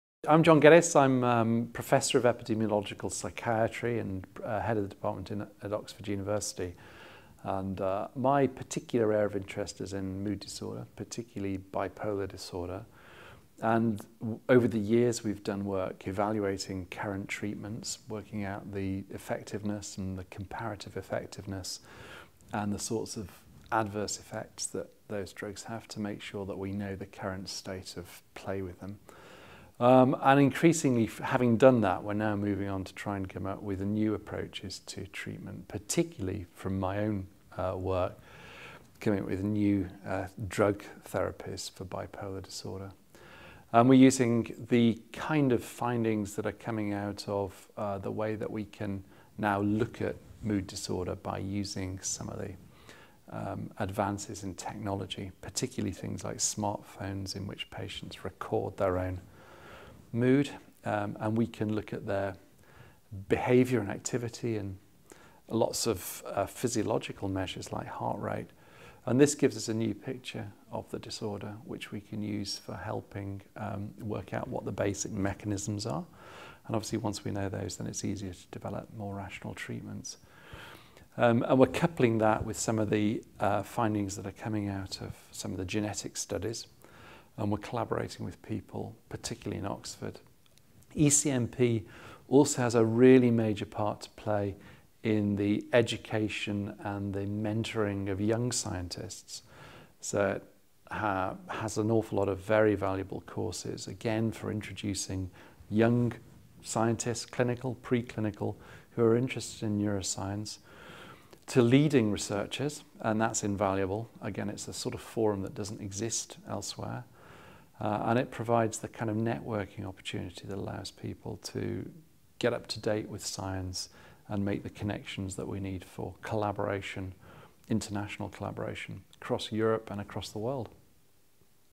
John Geddes and ECNP
John Geddes is a ECNP Neuropsychopharmacology Award winner. He gave a plenary lecture at the 29th ECNP Congress. In this interview he talks about his lecture 'Rediscovering drug discovery in bipolar disorder''  and ECNP.